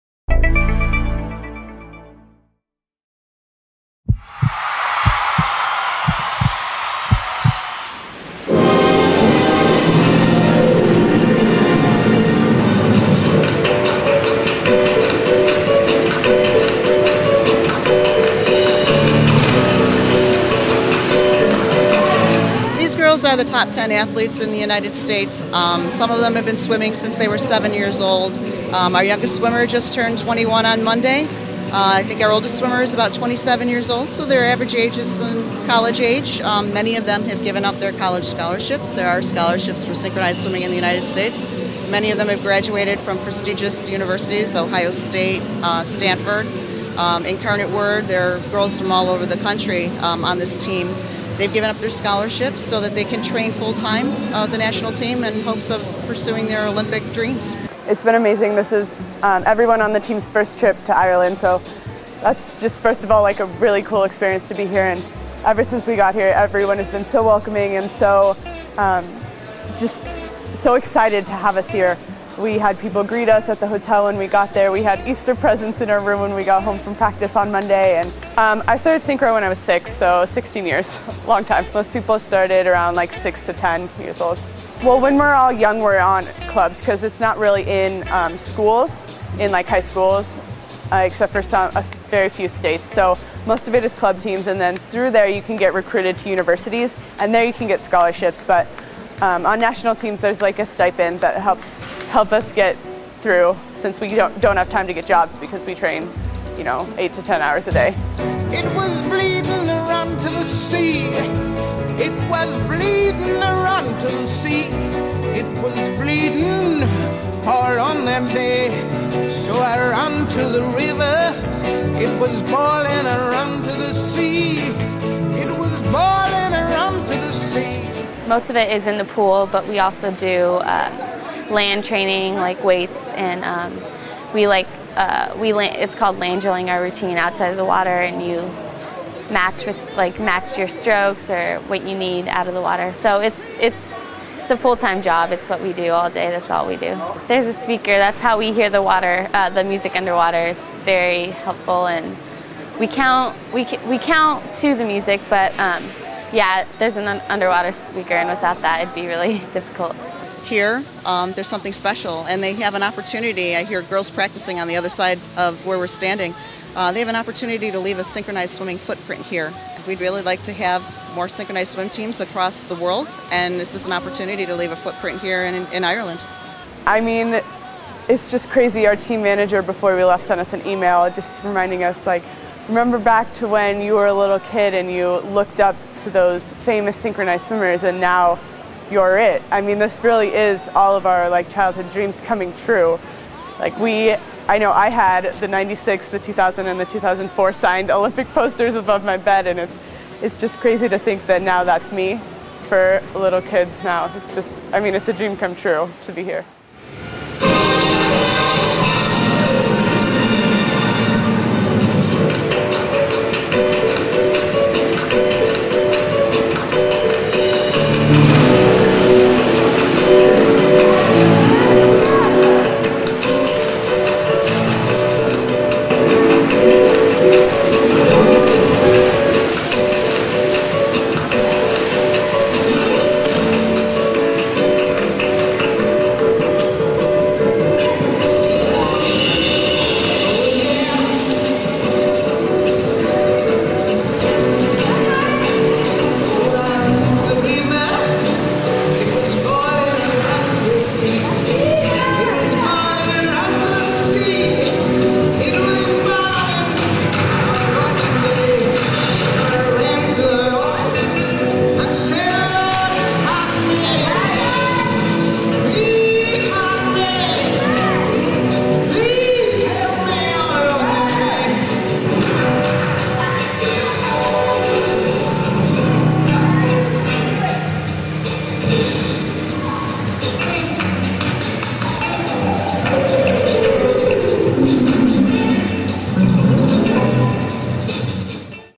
USA Synchronised Swim Team in Dublin
The USA Olympic Synchronized Swim Team had their first training session at the National Aquatics Center, Dublin on April 11, 2012. Team USA will be using Dublin as their base during July before departing for London and participating in the Olympic Games.